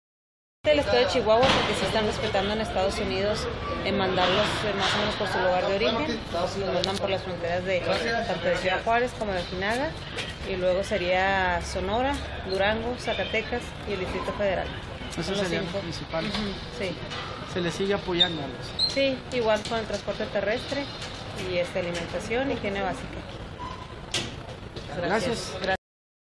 Aumenta paso de inmigrantes por Juárez
Aprovechan el clima para cruzar por esta frontera.